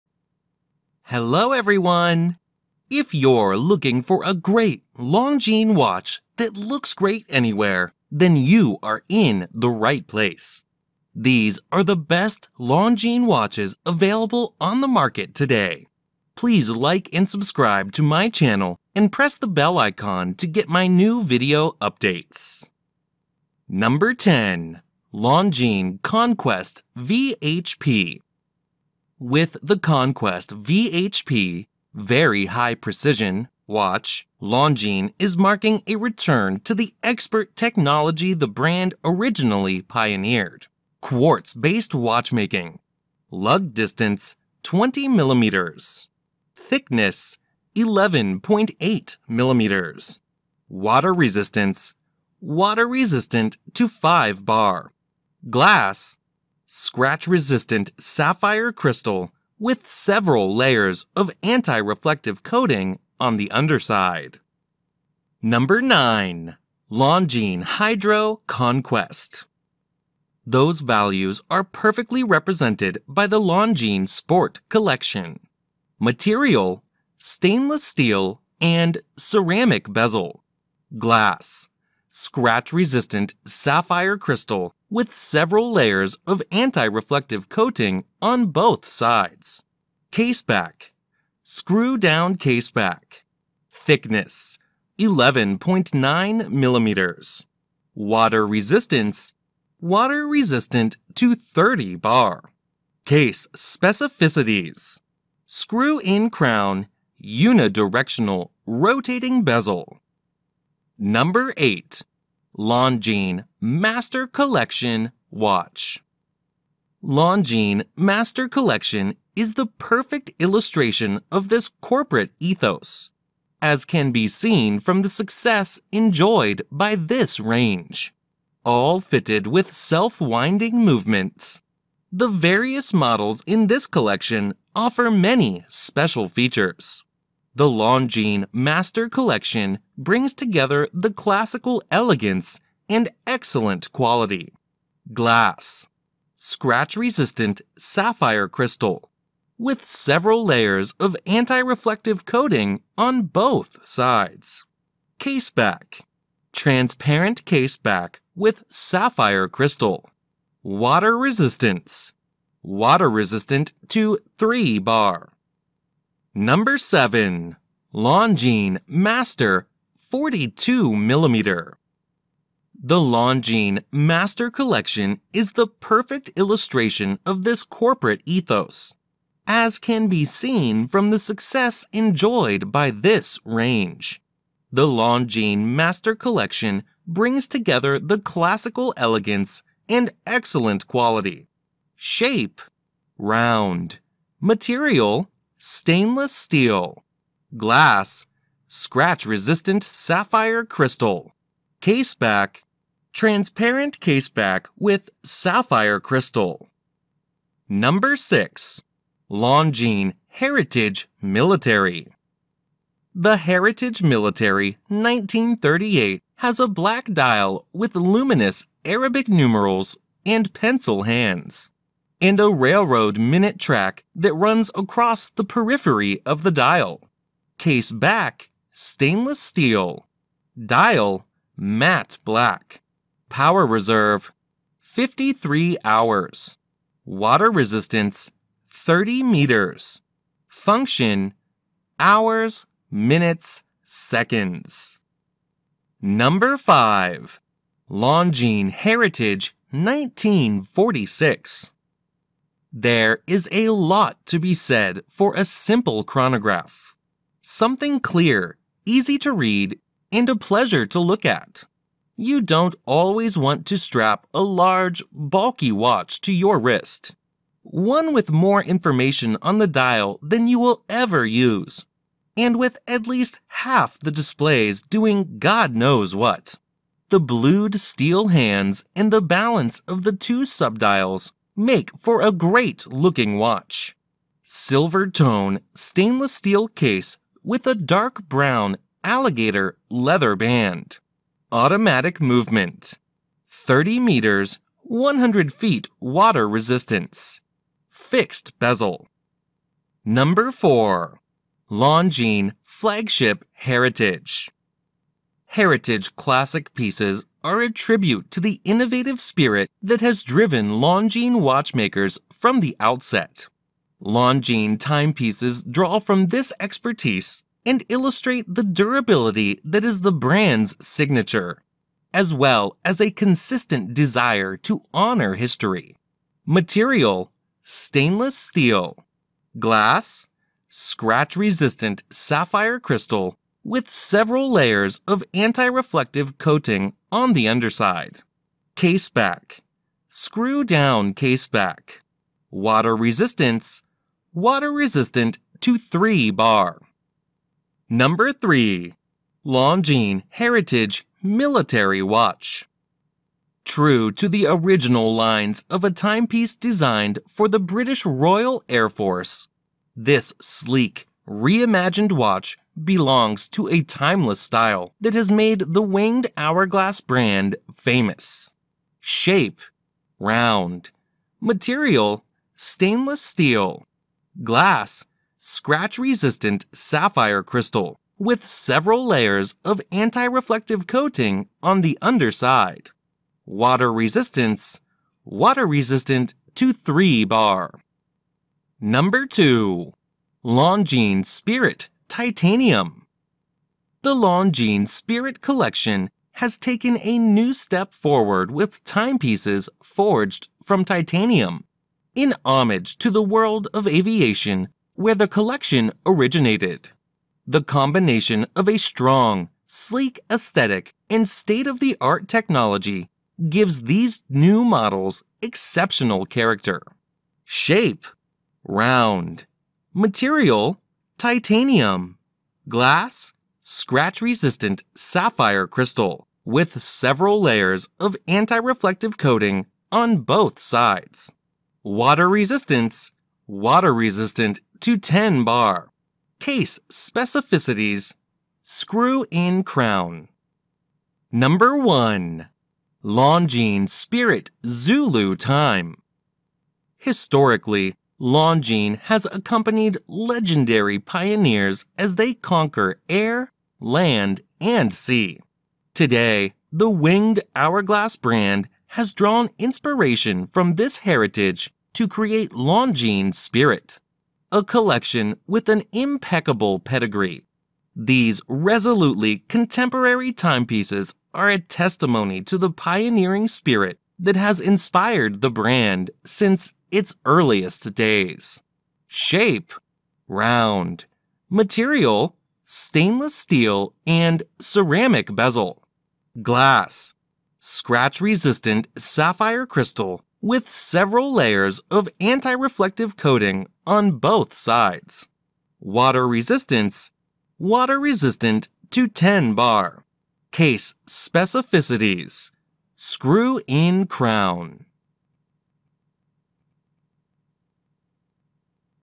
Top 10 Best New Longines Watches For Men Buy 2022
Are you looking for the best Longines watches for men on Amazon of 2022?
These are the best Longines Watches we found so far:

10.Longines Conquest V.H.P. Silver Dial Men's Watch

09.Longines HydroConquest Watch

08.Longines Master Collection Watch

07.Longines Master 42mm Collection Blue

06.Longines Longines Heritage Military 1938 Watch

05.Longines Heritage Classic Chronograph 1946 Watch

04.Longines Flagship Heritage

03.Longines Heritage Military Watch

02.Longines - Spirit Titanium

01.Longines Spirit Zulu Time